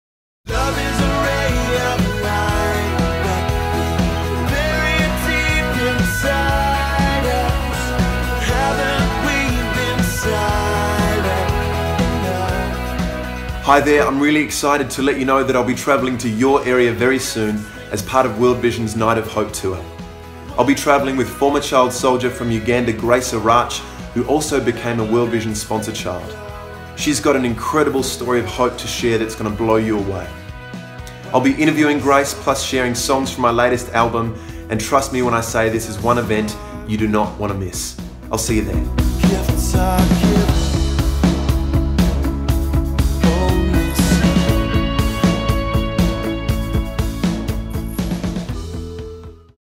A Night of Hope Tour 2016 (VIC & SA)
World Vision Australia is presenting A Night of Hope, a rare opportunity to hear the inspirational story of Grace Arach, former Ugandan child soldier, with performances by award-winning Australian singer/songwriter, Levi McGrath.

A Night of Hope will tour Victoria and South Australia, sharing the moving story of Grace, a former Ugandan child soldier who was abducted into the Lord’s Resistance Army (LRA) at the age of 12.

After enduring the horrors of war and becoming a child bride to a leader of the LRA, Grace made her dramatic escape and found refuge at a World Vision Child Soldier Rehabilitation Centre in Uganda.

In 2009, Levi McGrath and his wife Megan spent six life-changing months working with former child soldiers in Uganda through World Vision. Meeting Grace in Uganda greatly impacted Levi’s life and music.

A Night of Hope is an entertaining and moving experience with songs performed by Levi that were written about his time working at the Child Soldier Rehabilitation Centre, combined with Grace’s powerful story of survival and recovery.